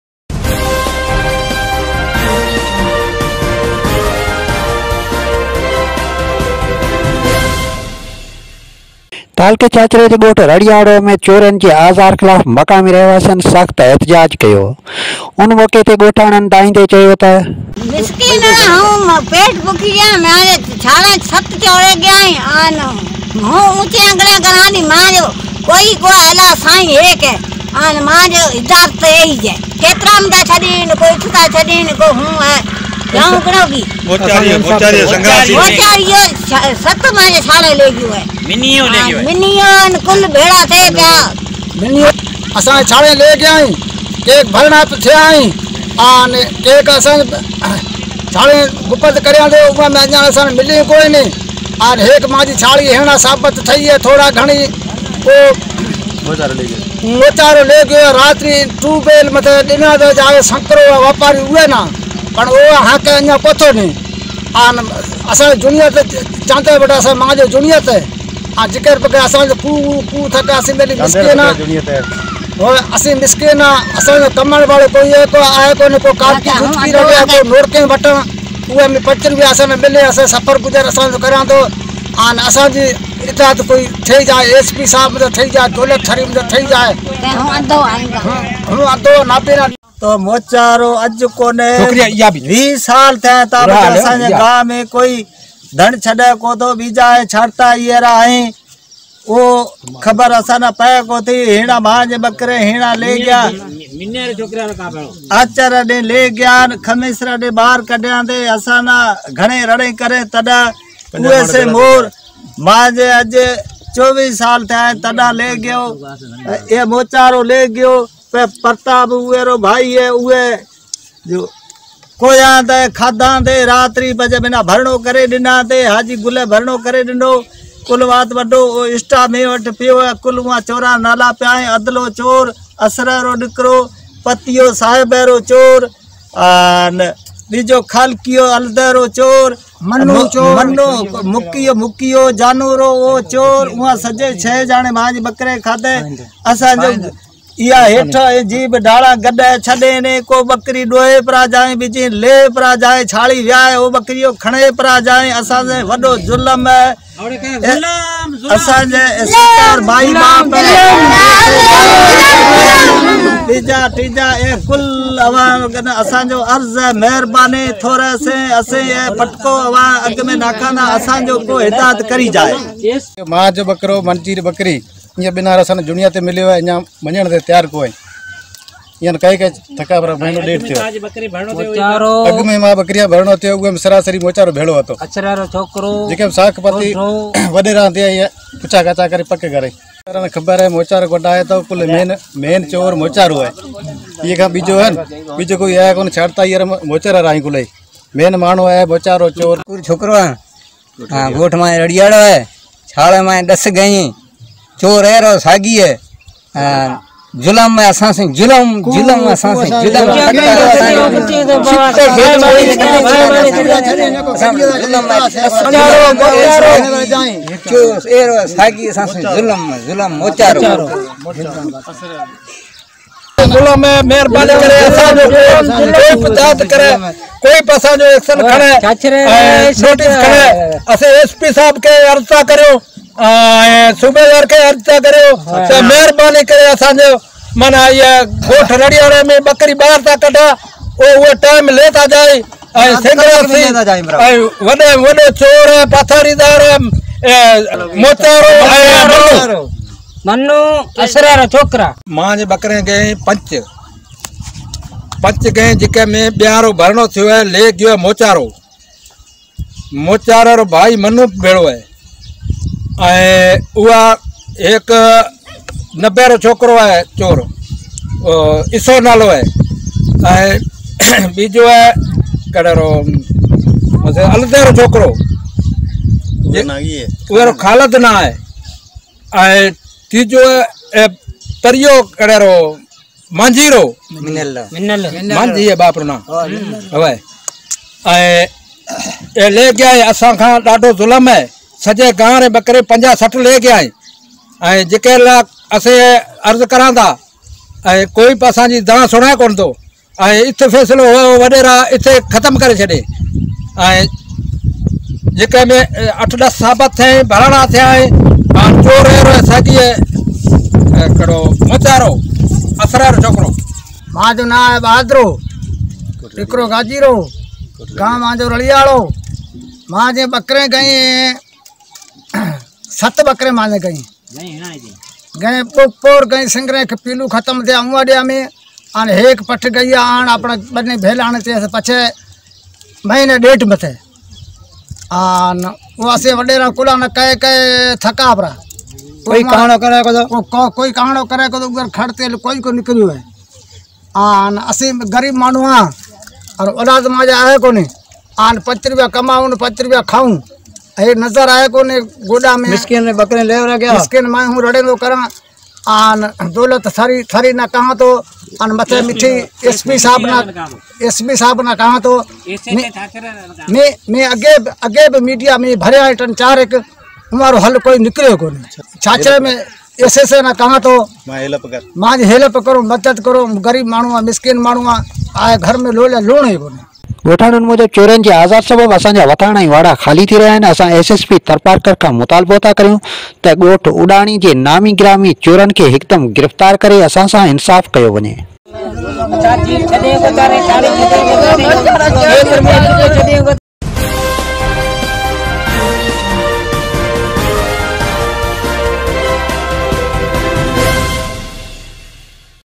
Chachro: Rariyaro Villagers Protest Against Thieves, When SSP Tharparkar Take Action Against Thieves
ٿرپارڪر: تعلقي ڇاڇري جي ڳوٺ رڙيارو ۾ چورين جي آزار خلاف مقامي رهواسي سراپا احتجاج، ڳوٺ اوڏاڻي جي نامي گرامي چورن خلاف ڪاروائي جو مطالبو